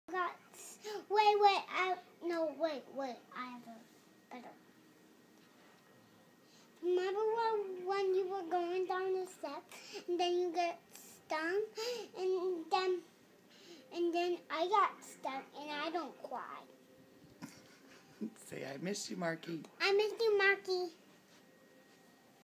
learning how to say things differently to be nice